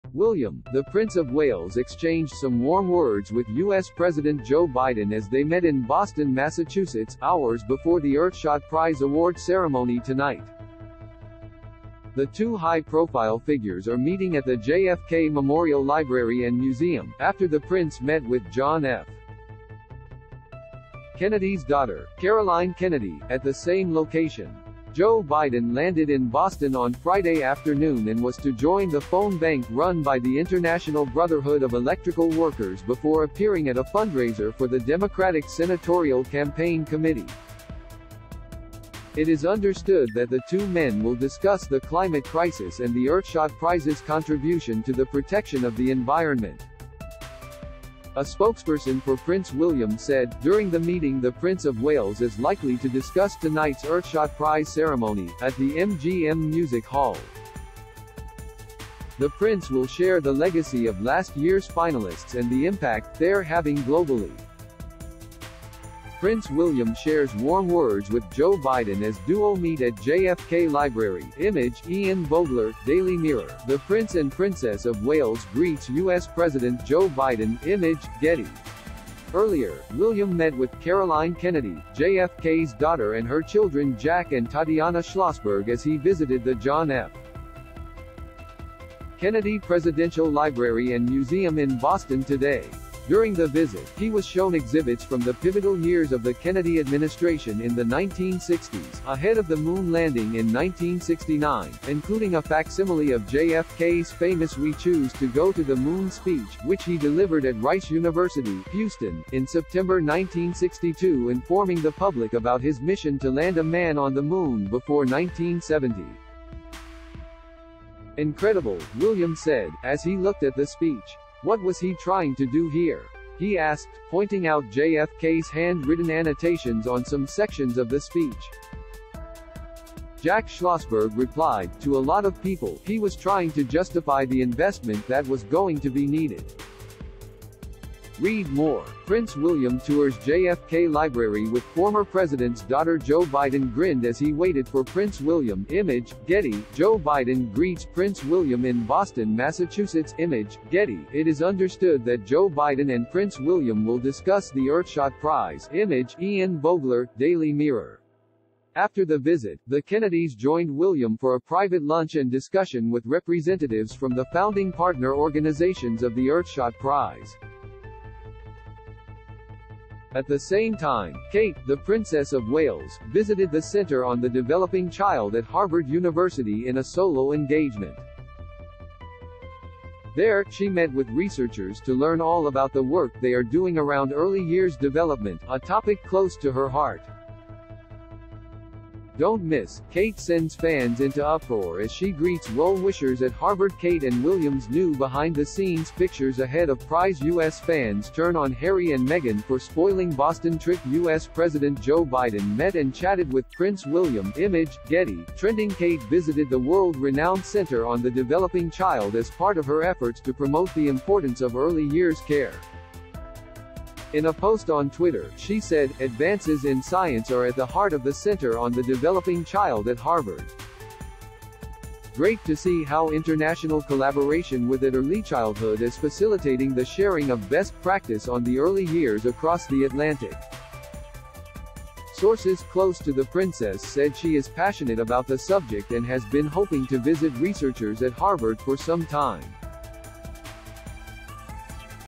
Prince William shares warm words with US President Joe Biden
William, the Prince of Wales exchanged some warm words with US president Joe Biden as they met in Boston, Massachusetts, hours before the Earthshot Prize Award ceremony tonight. The two high-profile figures are meeting at the JFK Memorial Library and Museum, after the Prince met with John F. Kennedy's daughter, Caroline Kennedy, at the same location.  Joe Biden landed in Boston on Friday afternoon and was to join the phone bank run by the International Brotherhood of Electrical Workers before a